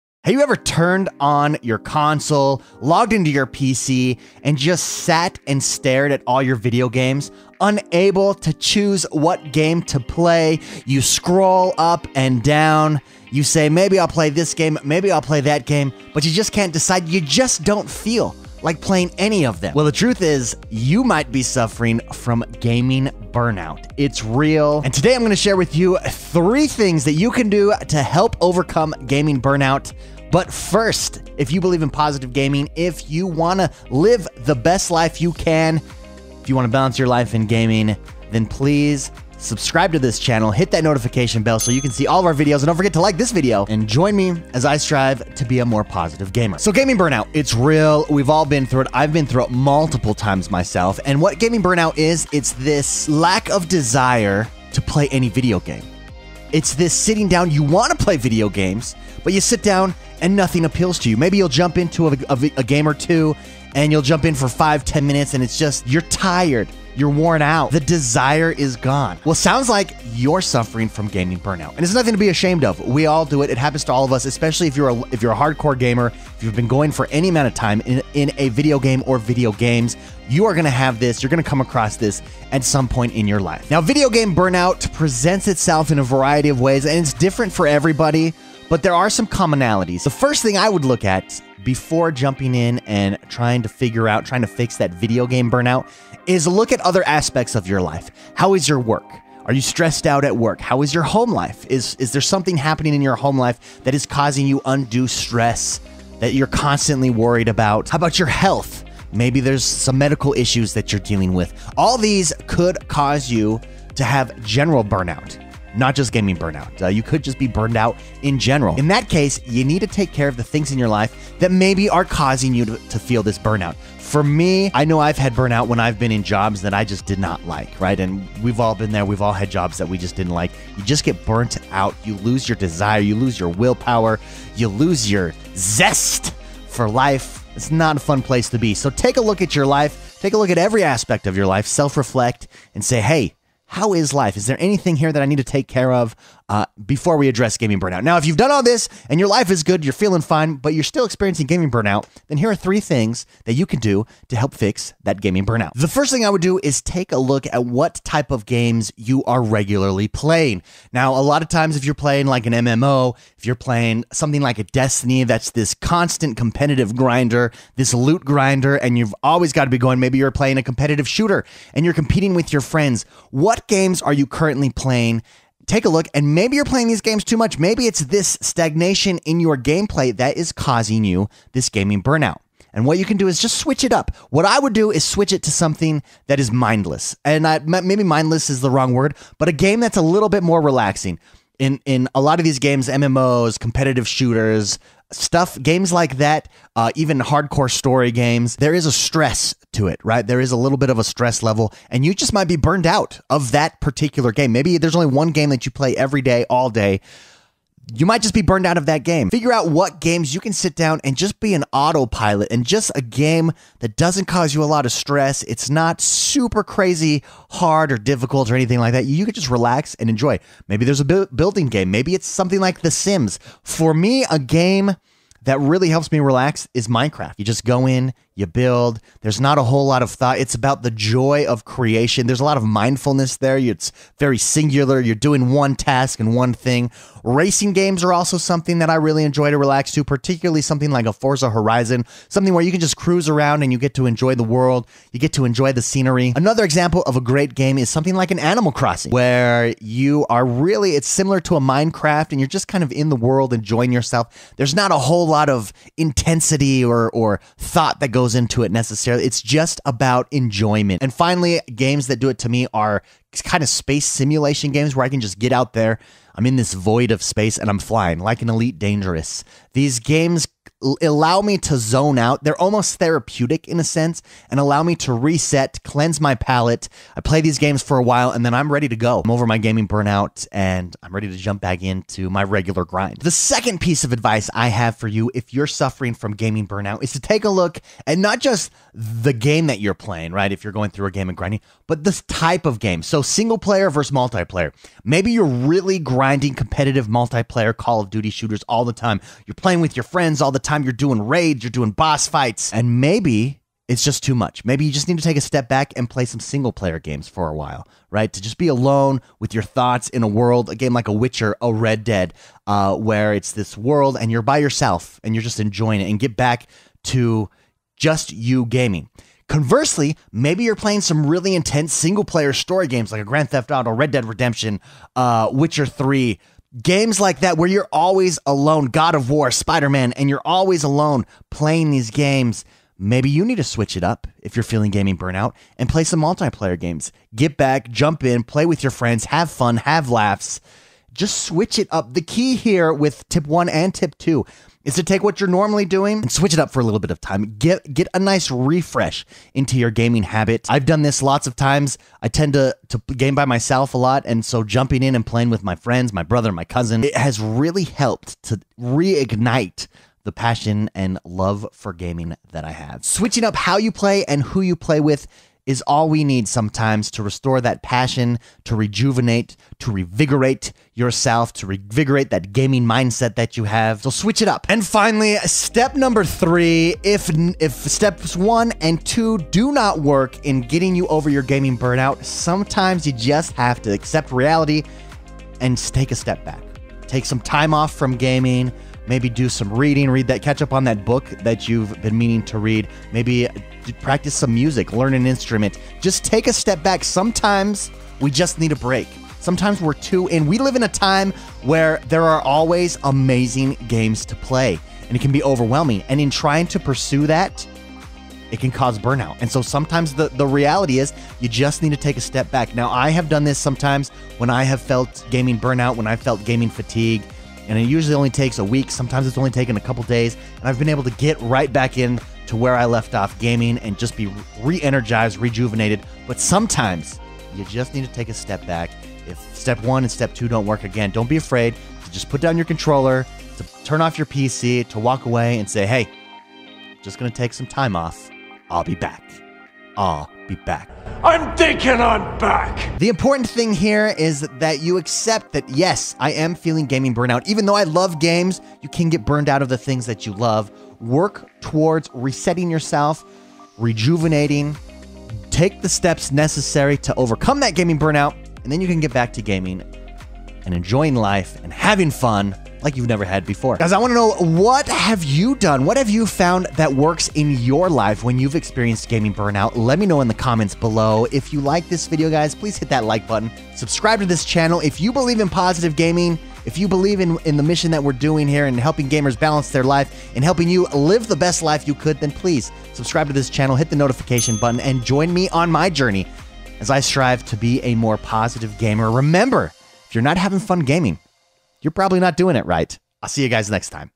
3 Ways to Overcome Gaming Burnout
We've all experienced it at some point. Here are 3 ways to overcome gaming burnout.

WE WANT TO HELP GAMERS LIVE THEY LIFE YOU DESERVE!